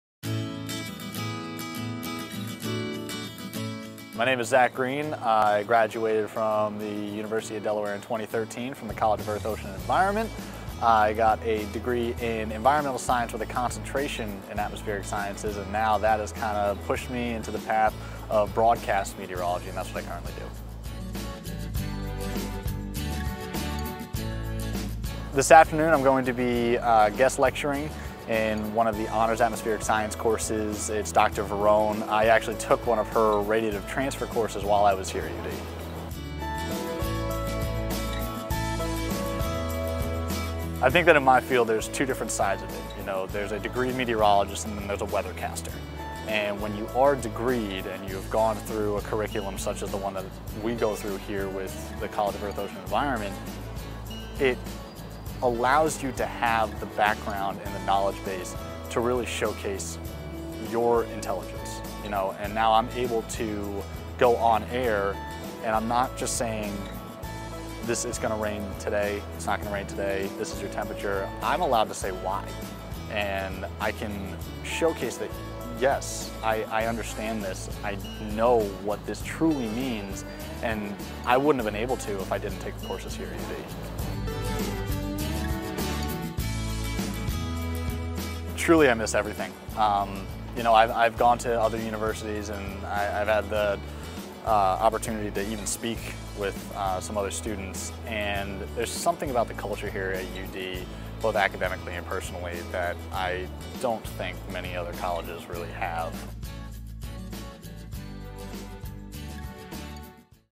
Meet Zack Green, Broadcast Meteorologist
Zack Green is a graduate of CEOE with a degree in Environmental Science and a Meteorologist in North Carolina with News 13.